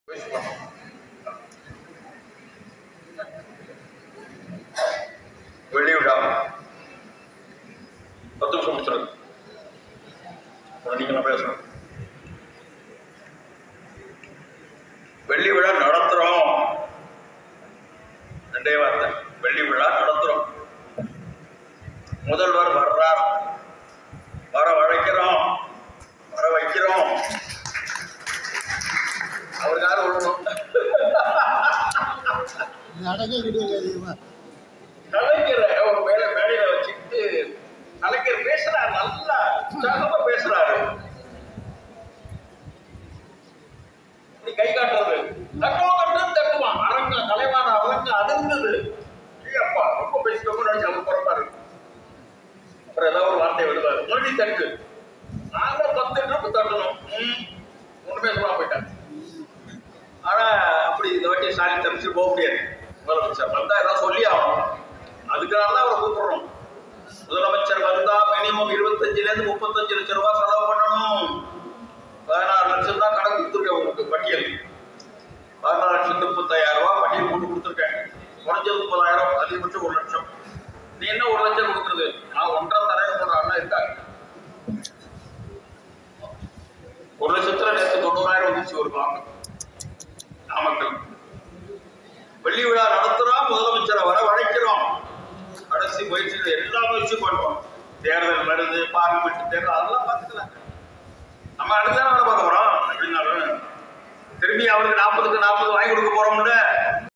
26.9.2023 Tanjore Dt.Papanasam Rtd.Officials Asscn.conducted State Peravai Function.
State Peravai Function conducted at Papanasam of Tanjore District.